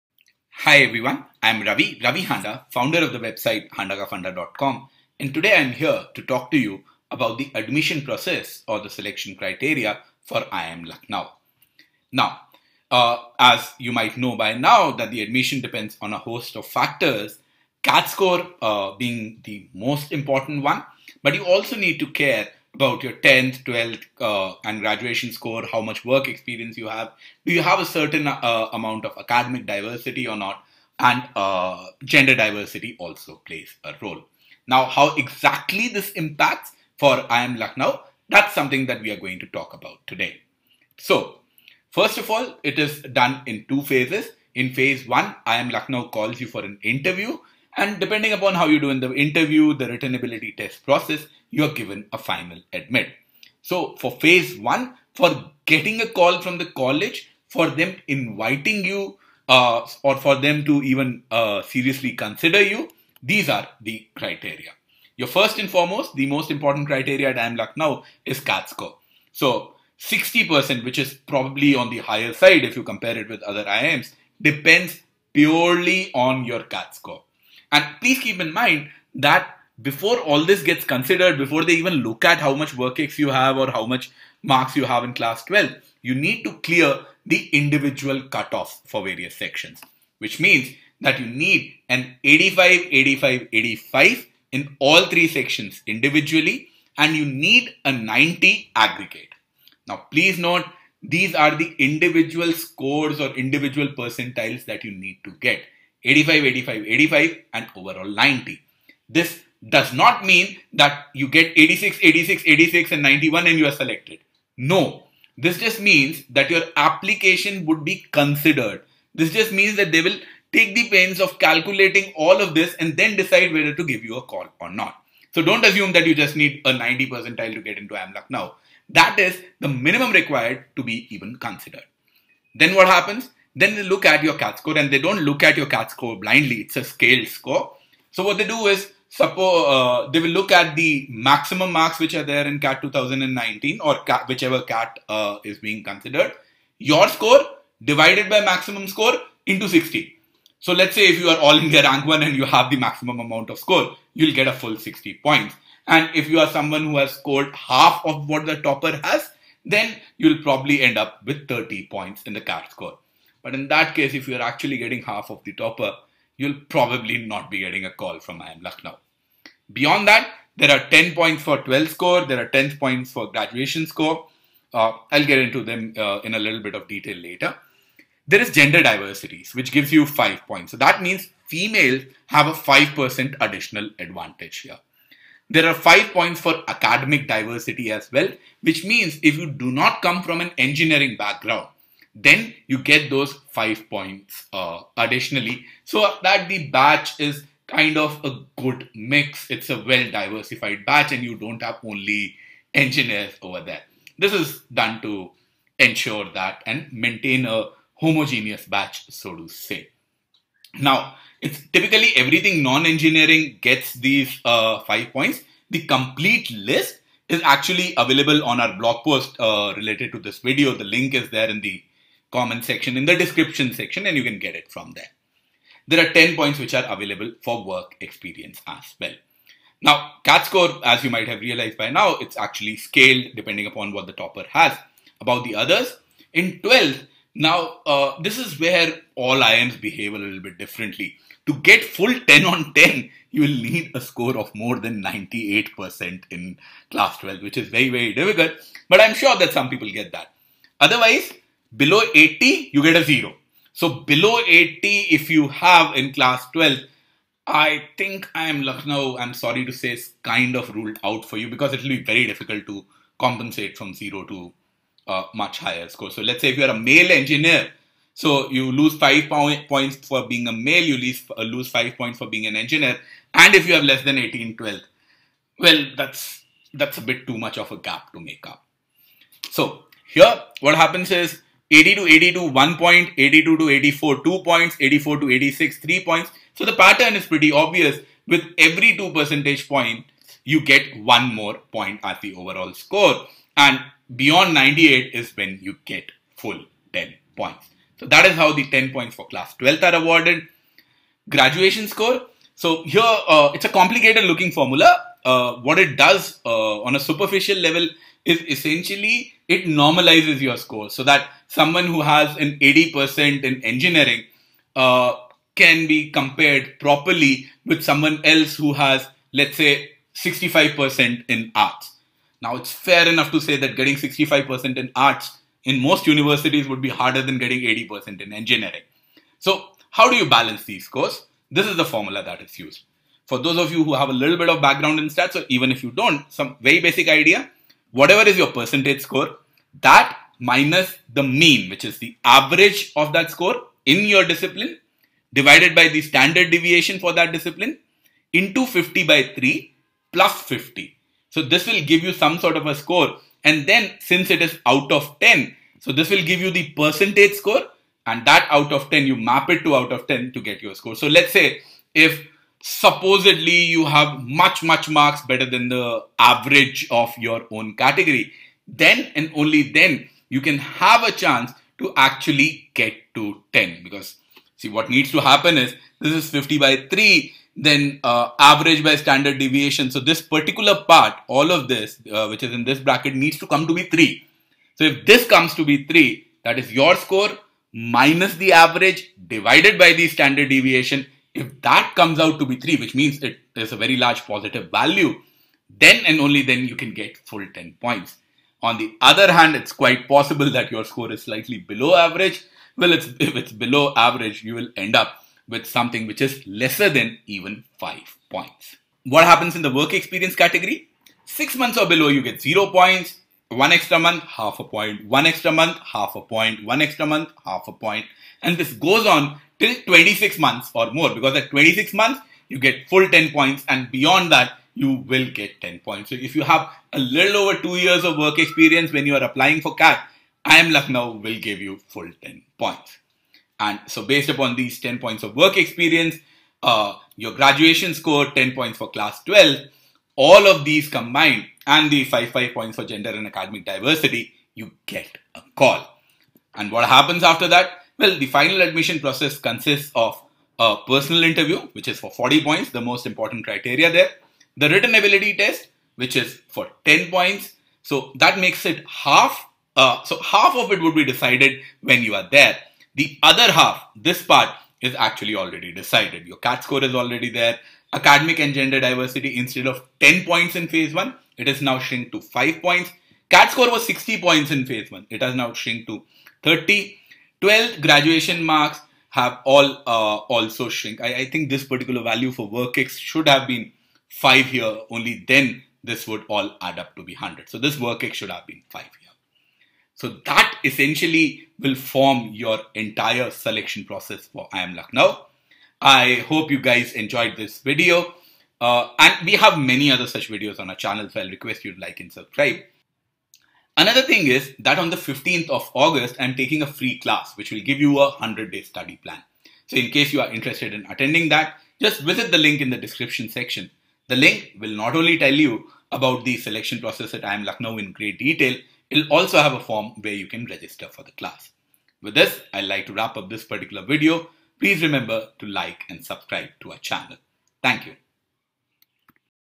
IIM Lucknow Admission Process / Selection Criteria for 2020-22 Batch
IIM Lucknow Admission Process:

The Indian Institute of Management Lucknow is an autonomous public business school in Lucknow, Uttar Pradesh, India.

IIM Lucknow Admission Process / Selection Criteria – Summary:

Whether you will get an interview call or admission from IIM Lucknow or not, it would depend upon the following factors (on a 100 point scale).

IIM Lucknow Admission Process / Selection Criteria – Complete Details:

The admission process for the PGP program comprises two stages.

1. Shortlisting candidates for WAT/PI round.
2. Final Selection of candidates post WAT/PI round

For Complete Details of IIM Lucknow Admission Process / Selection Criteria